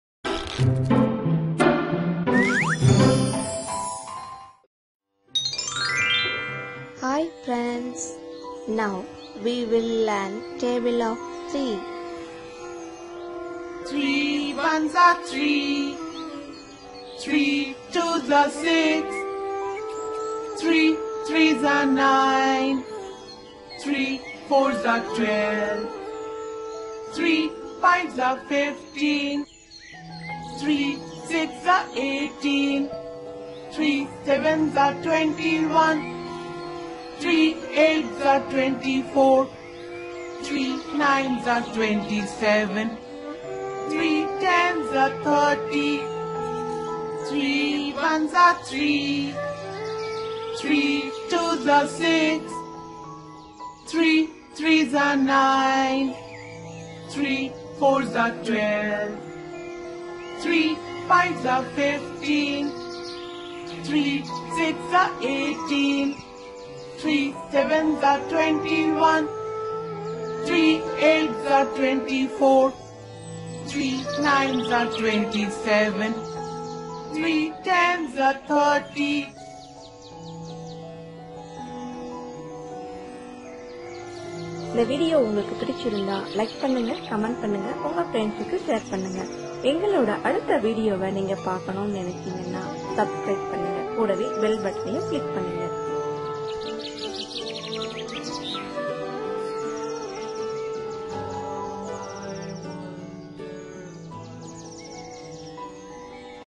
Learn Multiplication Table Of Three 3 x 1 = 3 | 3 Times Tables | Fun & Learn | KidsTv Sirukathaigal
Hi all

Fun & Learn Multiplication Table Of THREE, Easy way to Learn Tables of "3" For Your Children with Fun And Learn Videos Best Multiplication table songs